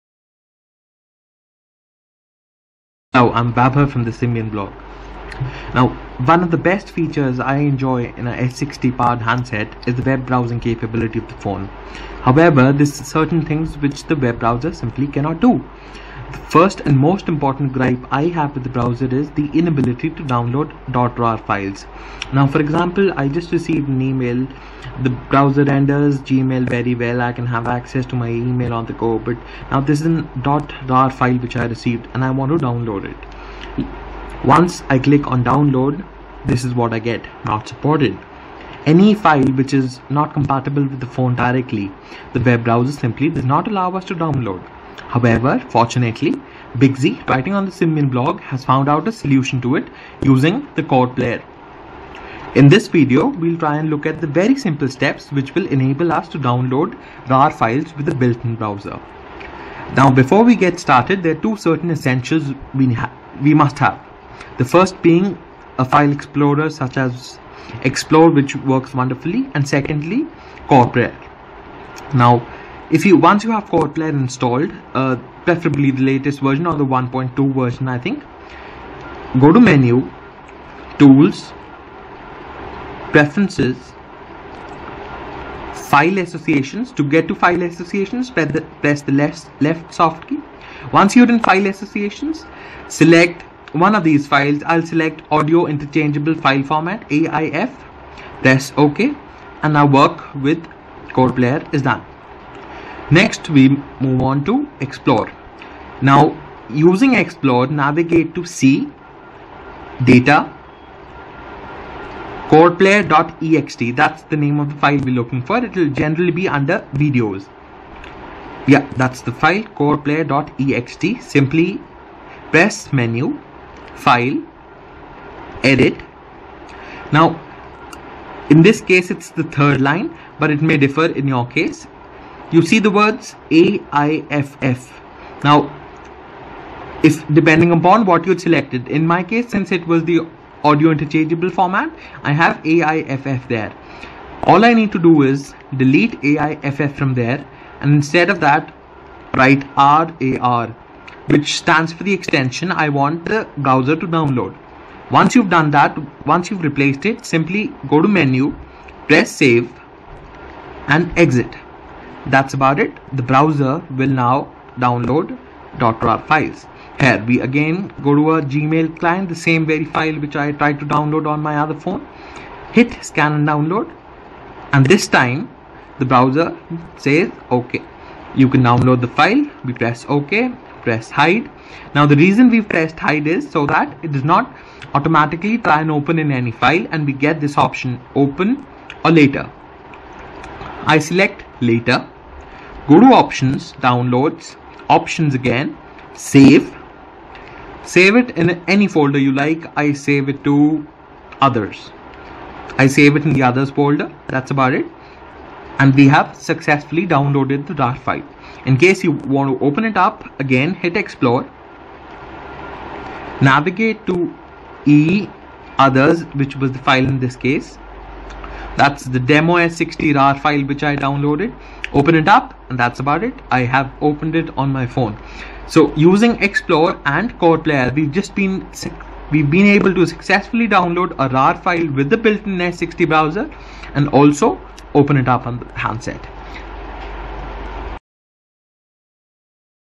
How To Download .RAR Files With The S60 Browser
Using Coreplayer and Xplore we tweak the phone to allow us to download .RAR files from the default S60 Browser.